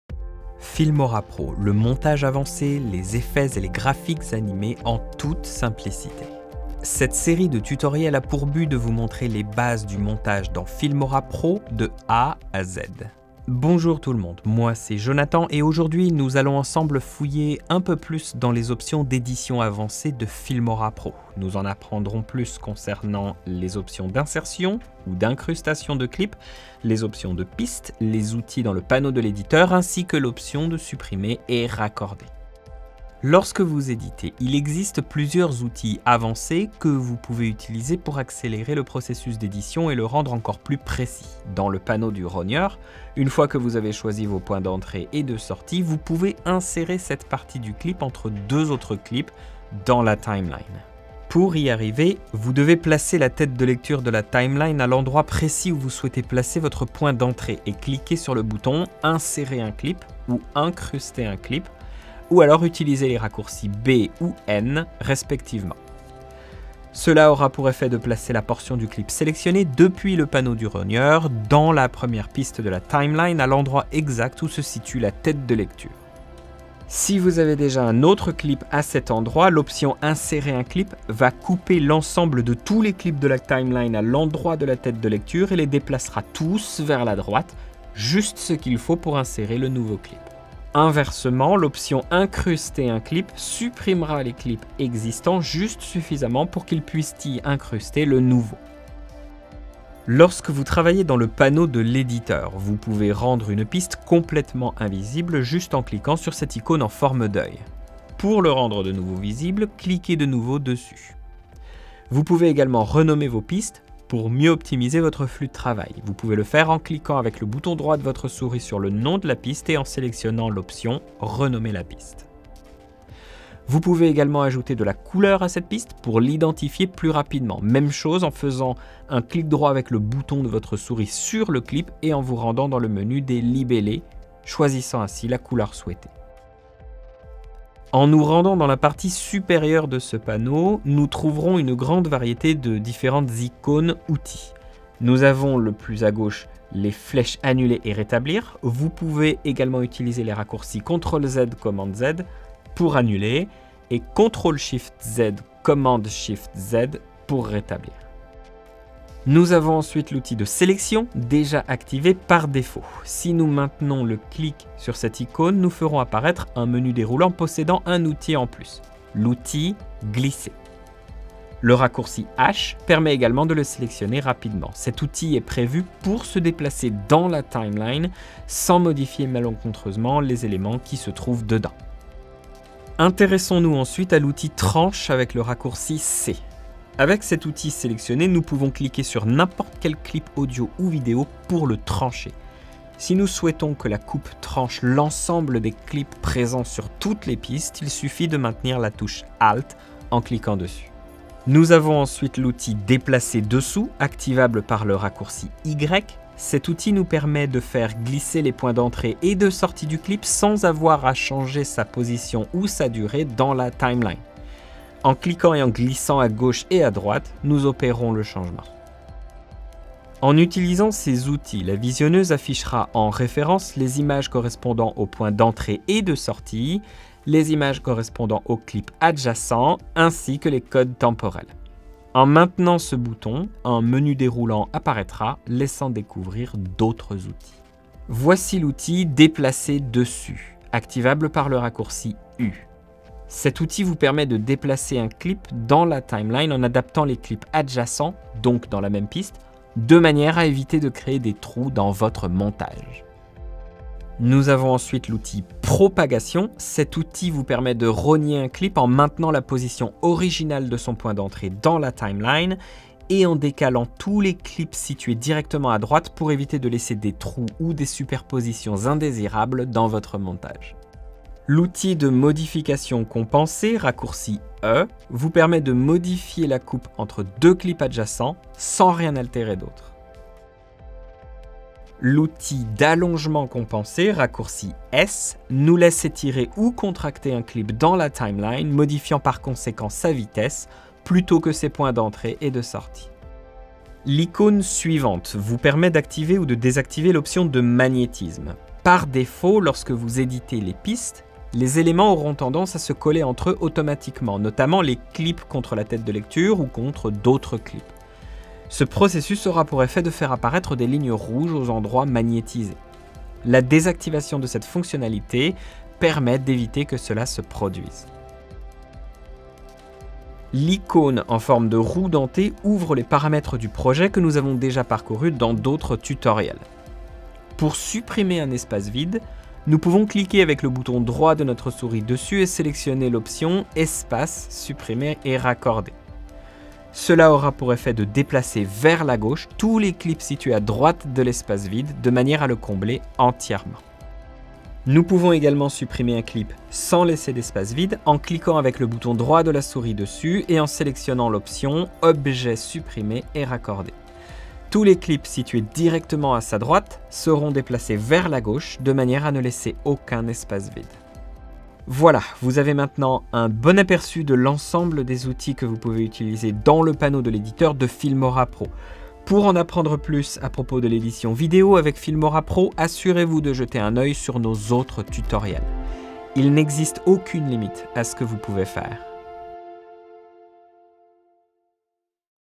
🎬Introduction à l'édition avancé|| TUTO FilmoraPro-💪🏻Maitriser Montage Vidéo #10
Notre série habituelle pour démarrer facilement avec FilmoraPro

Cette série de tutoriels permettent même aux plus débutants d'entre-vous d'appréhender facilement le logiciel de montage professionnel FilmoraPro.

L'objectif ici est de vous permettre de commencer rapidement à construire un projet et d'être capable d'utiliser toutes les ressources de base que propose le logiciel.

Cette série est donc faite pour vous accompagné dans votre aventure d'apprentissage.

N'hésitez pas à nous dire si vous avez des suggestions dans les commentaires.